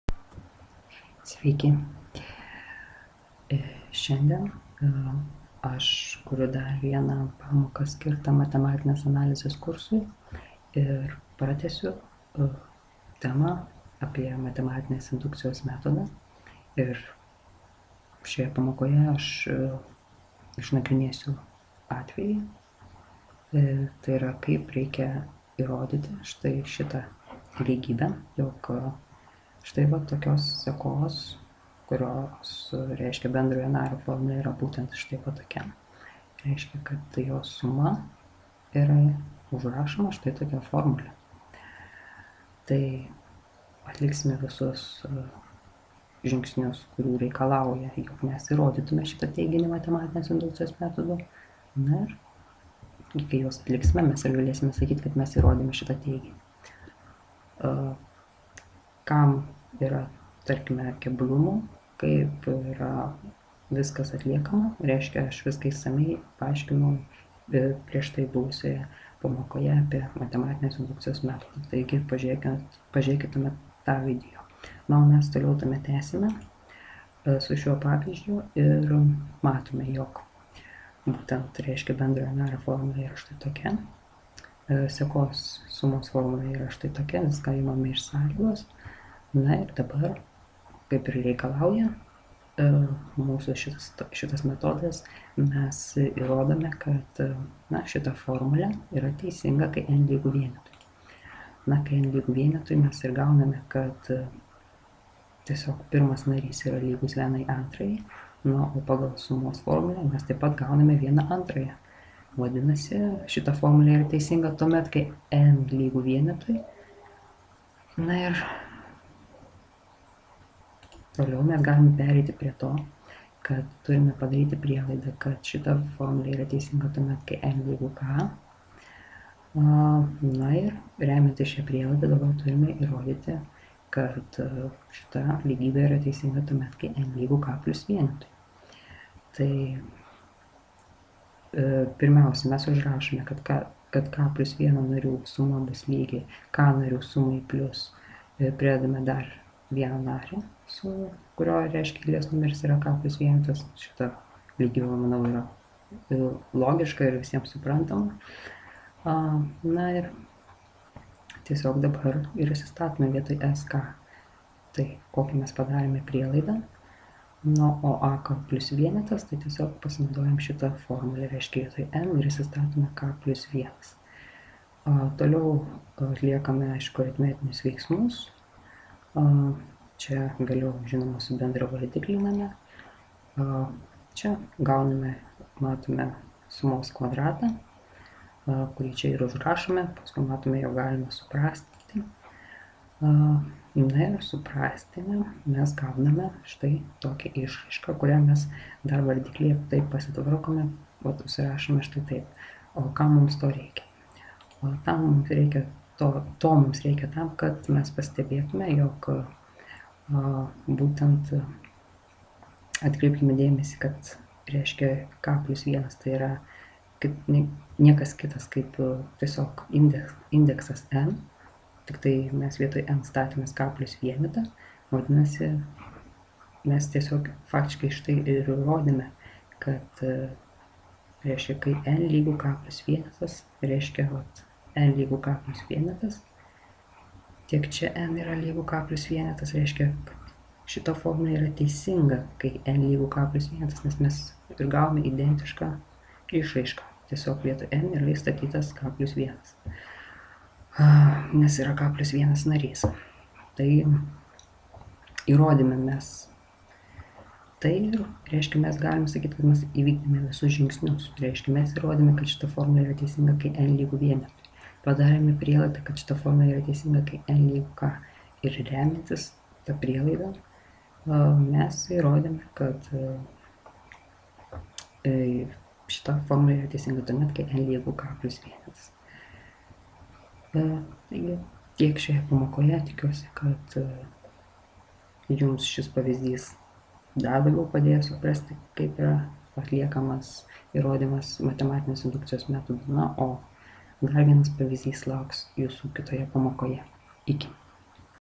Teiginių įrodymas matematinės indukcijos metodu (2 dalis)
Sveikinuosi su tais, kurie mokosi matematinės analizės kursą. Šioje pamokoje tęsiu aiškinimą apie matematinės indukcijos metodo taikymą įrodant teiginius. Tikiuosi ši pamoka bus naudinga, iki susitikimo ;)

P.S. Nepagailėkite mygtukų "Patinka", jie mane tikrai labai nudžiugina ir įkvepia
kurti pamokas jums toliau. Nepamirškite ir užsiprenumeruoti šio kanalo kas dar to
nepadarė :)